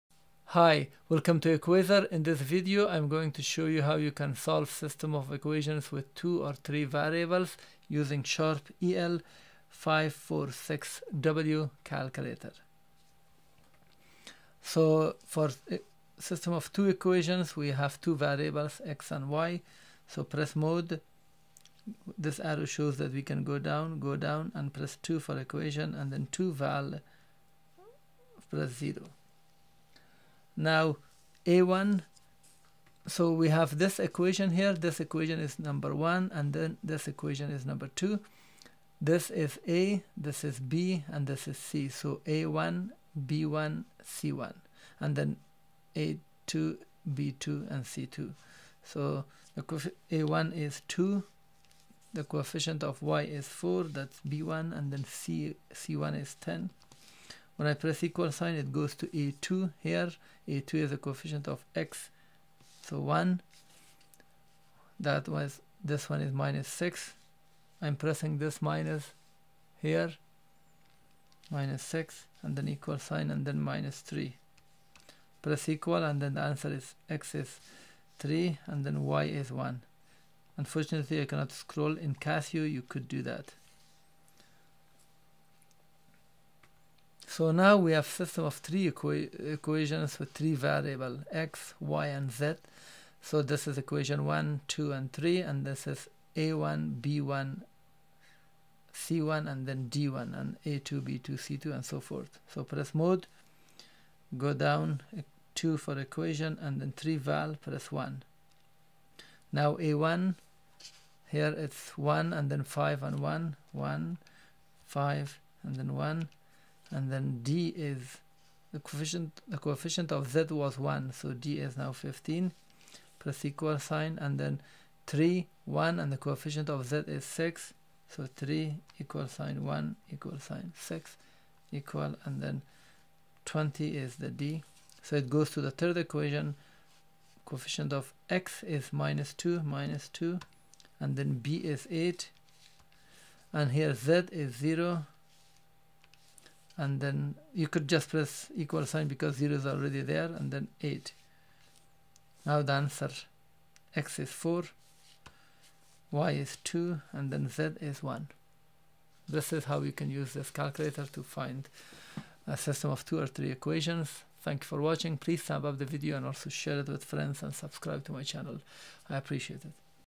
Sharp EL-546W EL506W calculators solving system of 2 and 3 equations
this video shows you how to solve system of 2 or 3 equations with 2 and 3 variables using Sharp EL-546W EL506W calculators.
Tutorial by Equaser.